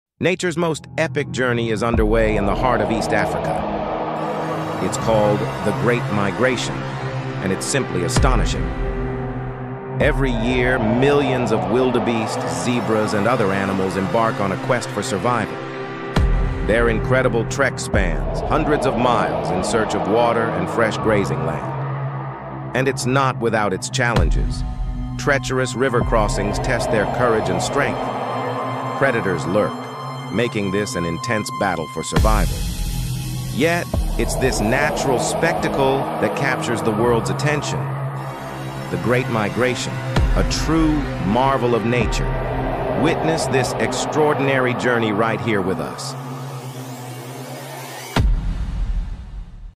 The Great Migration.
"Experience the awe-inspiring journey of The Great Migration, an annual wildlife spectacle in East Africa. Watch millions of wildebeest, zebras, and other animals traverse vast landscapes in search of water and fresh grazing land. Witness the circle of life and the challenges of nature in action. 🦓🌍 GreatMigration WildlifeSpectacle AfricanSafari AnimalMigration EastAfricaSafari NatureDocumentary"